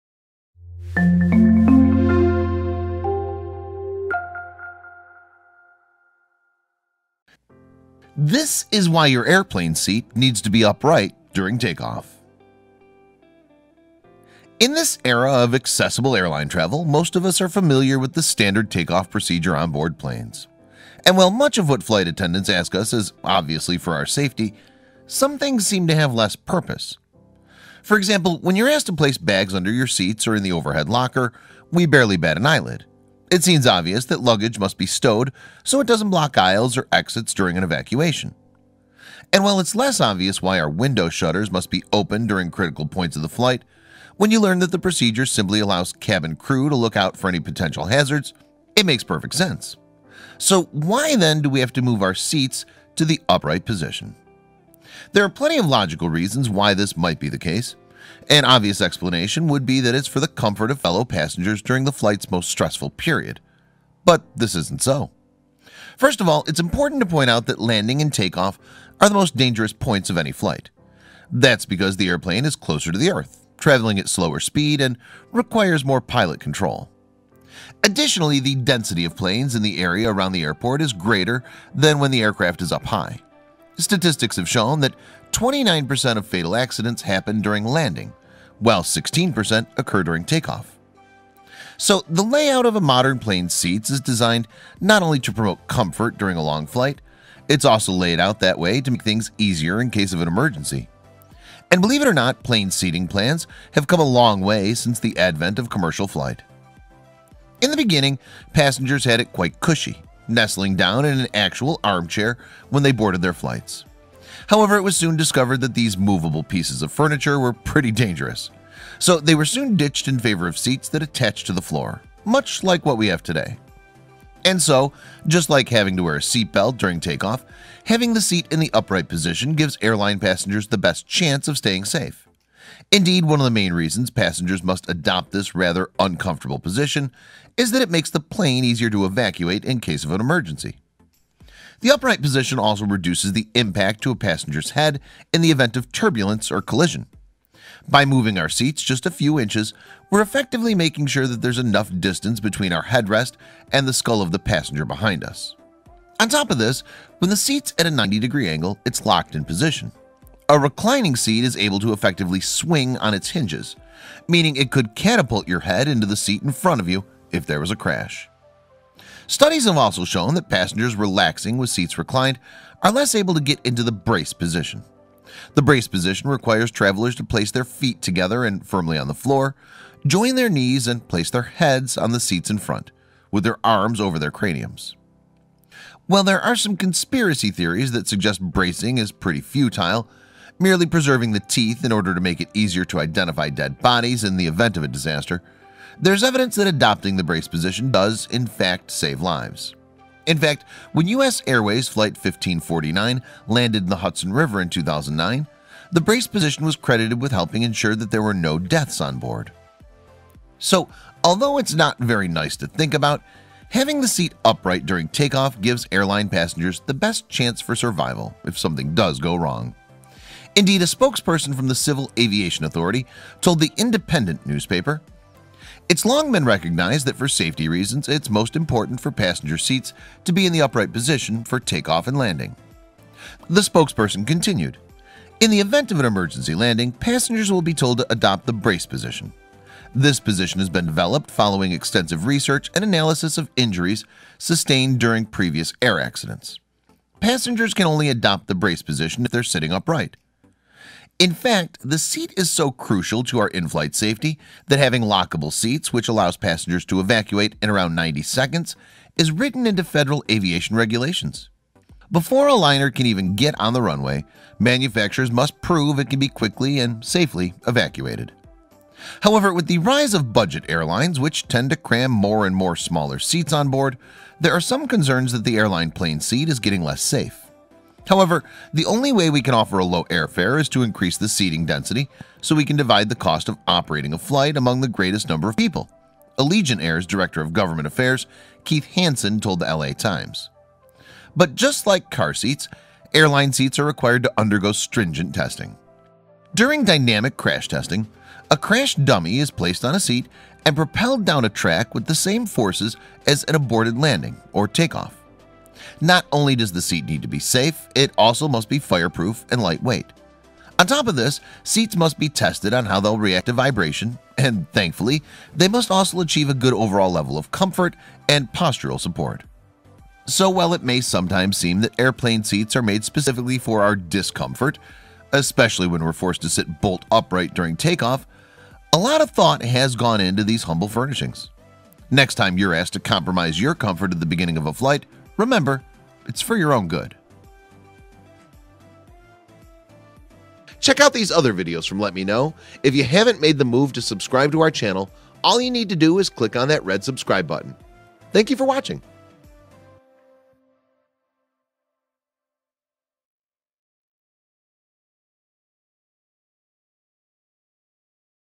This Is Why Your Airplane Seat Needs To Be Upright During Takeoff
In this era of accessible airline travel, most of us are familiar with the standard takeoff procedure onboard planes. And while much of what flight attendants ask of us is obviously for our safety, some things seem to have less purpose.